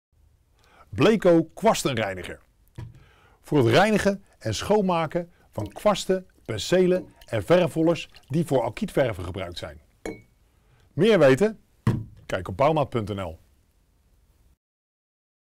Bleko - Kwastenreiniger
De video toont een snelle review van de Bleko kwastenreiniger en hieronder is nog extra informatie te vinden.

Dit product is geschikt voor alkydverven en vrij van schadelijke aromaten. Uiteraard is kwastreiniger te gebruiken voor het reinigen en schoonmaken van bijvoorbeeld kwasten, penselen en verfrollers welke gebruikt zijn voor alkydverven.